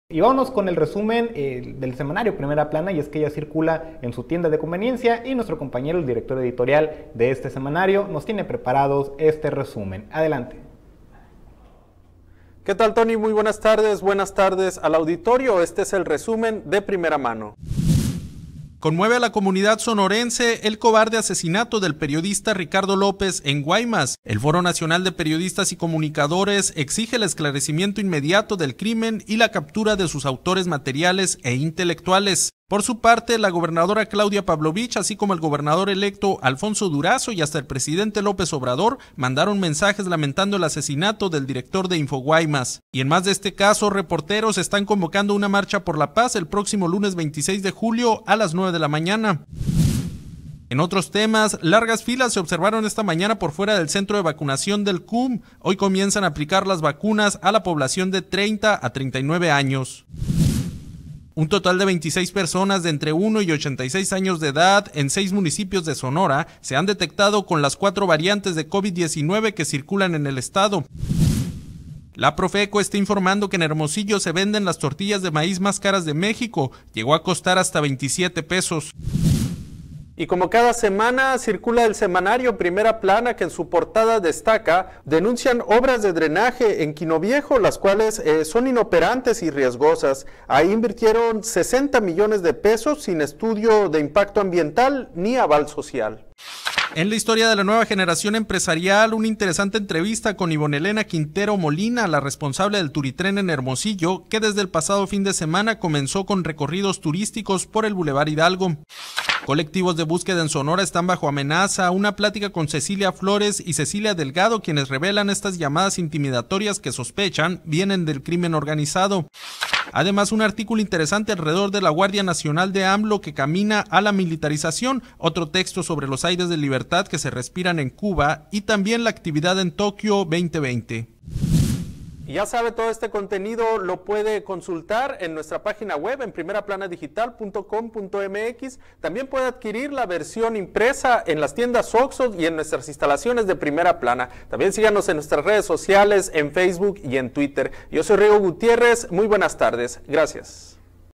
Resumen “De Primera Mano”…
📍Conmueve😢 a la comunidad sonorense el cobarde asesinato del periodista Ricardo López en Guaymas

📍El Foro Nacional de Periodistas y Comunicadores📻📹🎙️exige el esclarecimiento inmediato del crimen y la captura de sus autores materiales e intelectuales

📍Por su parte, la gobernadora Claudia Pavlovich, así como el gobernador electo, Alfonso Durazo, y hasta el presidente López Obrador, mandaron mensajes👍lamentando el asesinato del director de InfoGuaymas

📍Y en más de este caso, reporteros están convocando a marcha por la paz🕊️el próximo lunes 26 de julio a las nueve de la mañana

📍En otros temas, largas filas se observaron esta mañana por fuera del centro de vacunación💉del CUM. Hoy comienzan a aplicar vacunas a la población de 30 a 39 años

📍Un total de 26 personas, de entre 1 y 86 años de edad, en seis municipios de Sonora, se han detectado con las cuatro variantes de COVID-19😷que circulan en el Estado

📍La Profeco está informando que en Hermosillo se venden las tortillas de maíz🌽más caras de México, llegó a costar hasta 27 pesos